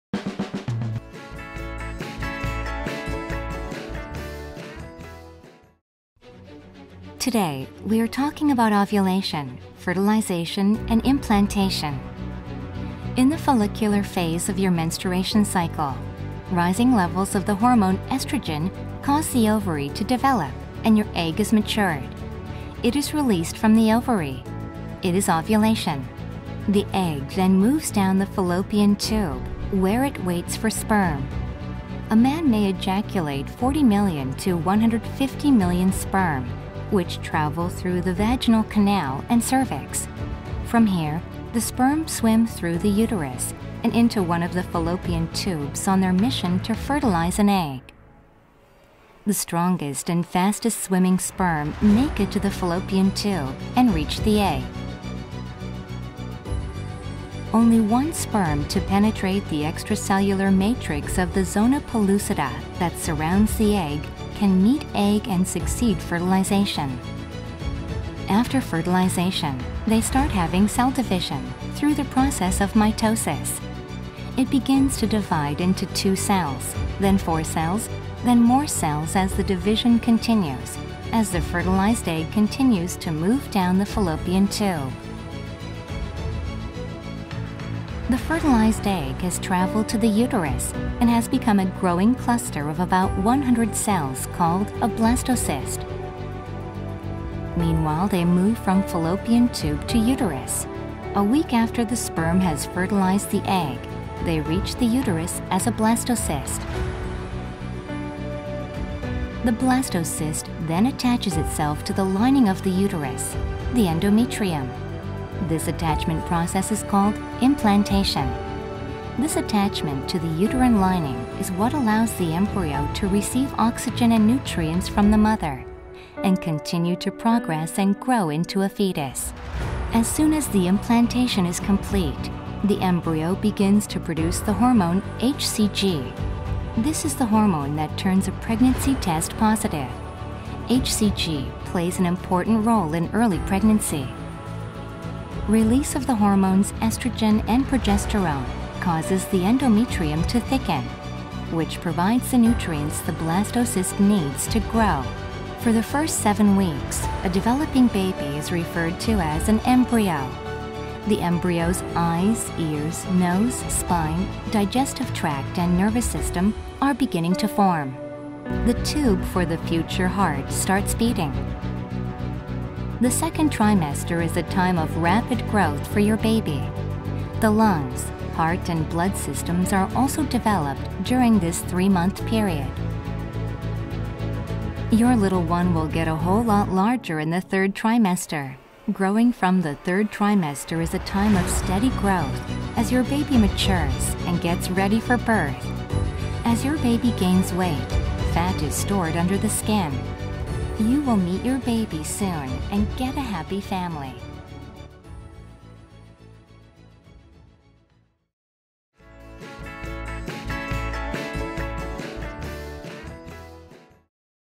Pregnancy, The process of Ovulation, Fertilization and Implantation
This shows the process of ovulation, fertilization and implantation in a woman's womb. The video also includes animations of the fetal development